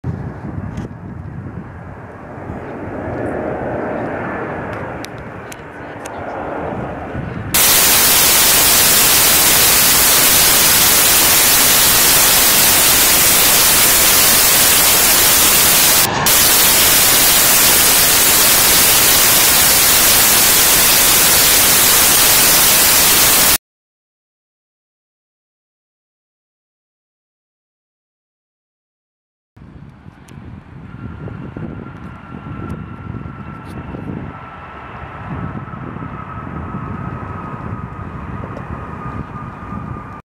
Helvetic Airlines Embraer ERJ-190LR (HB-JVQ) landing @Zurich [03.01.2016 - 11:02]
Flight LX1017 - Dusseldorf (DUS) to Zurich (ZRH)